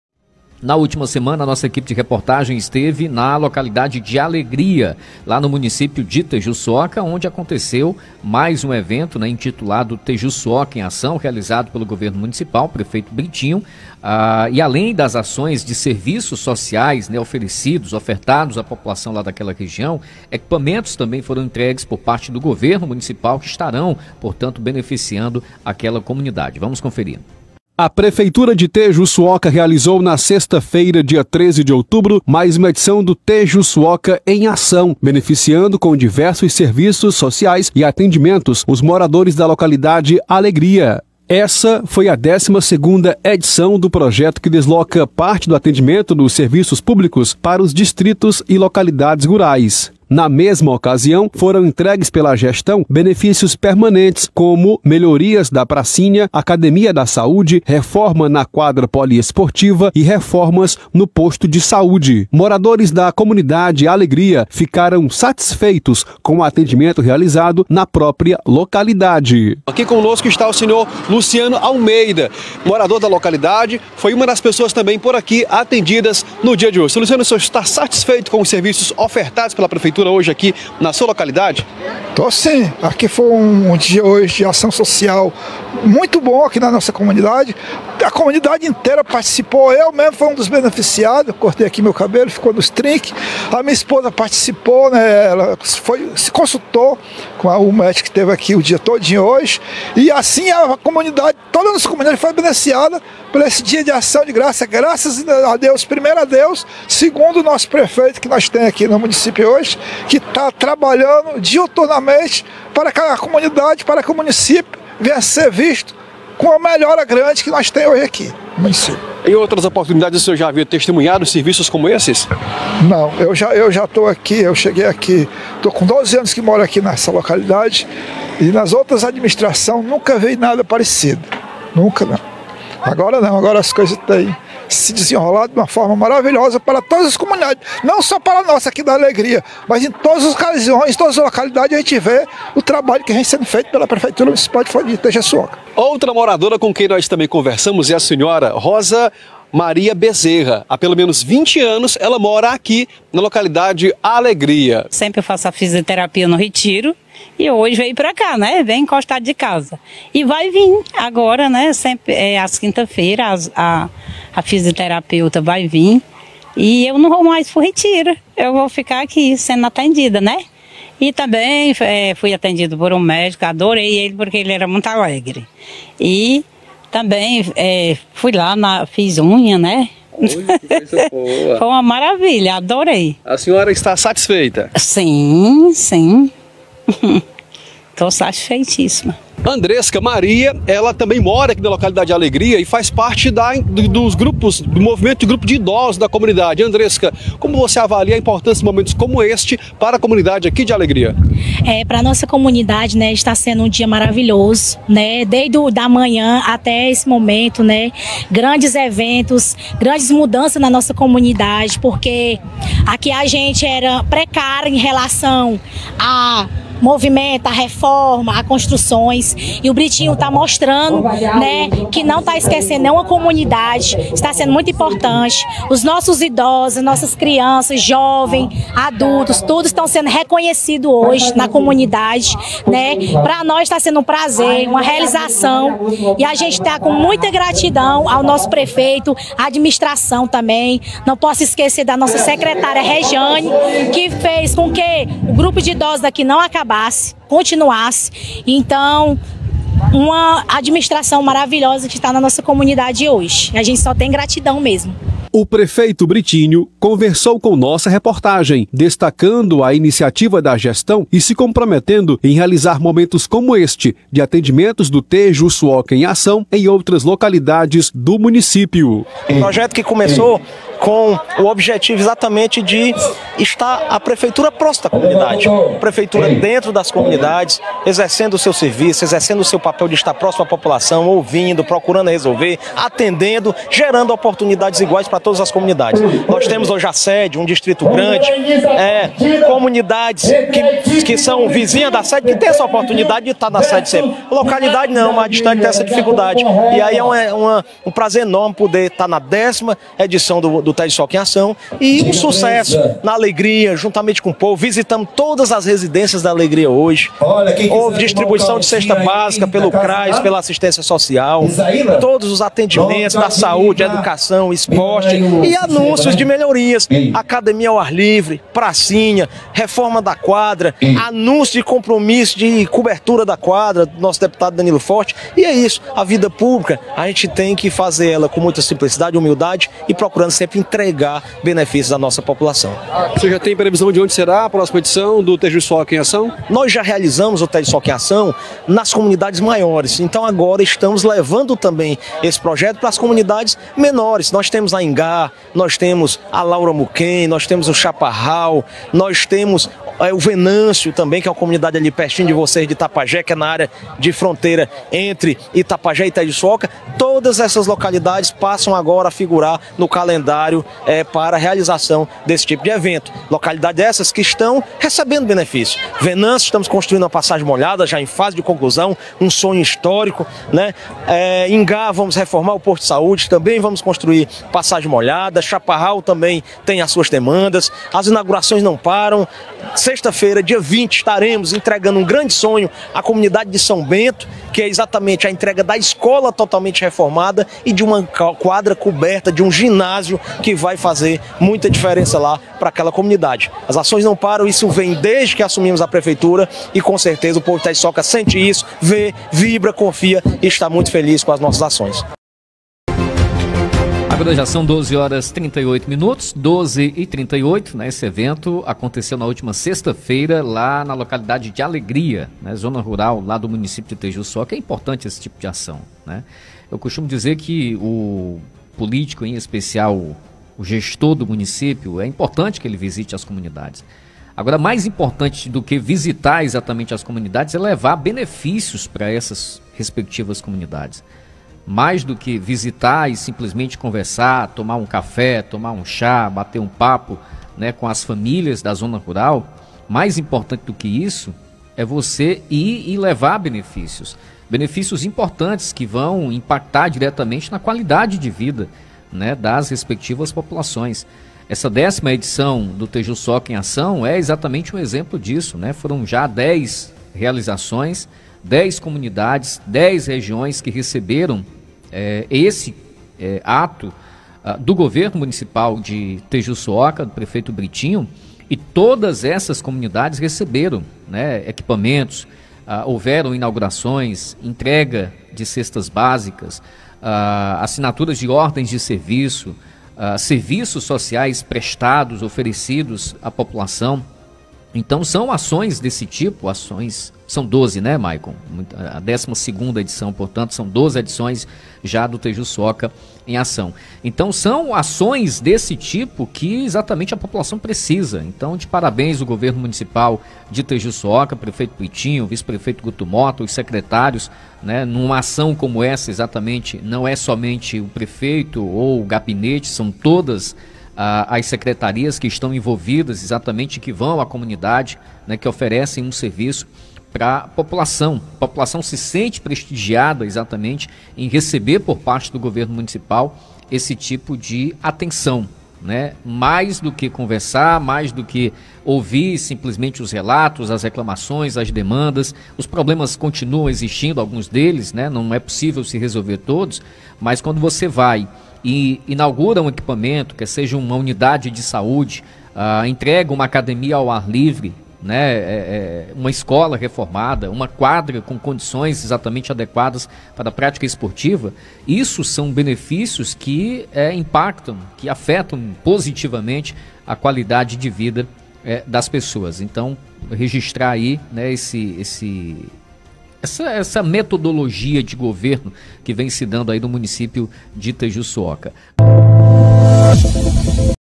Tejuçuoca: Governo Municipal entrega equipamento na localidade de alegria. 17/10/2023
O Governo municipal de Tejuçuoca realizou, na última sexta-feira (12), a 12ª edição do Tejuçuoca em ação. O projeto leva às comunidades, serviços sociais e demais benefícios permanentes desenvolvidos pela atual gestão. Desta feita, a comunidade contemplada foi a de alegria, na zona rural do município. Nossa equipe de reportagem esteve no local acompanhando o momento e ouviu alguns presentes.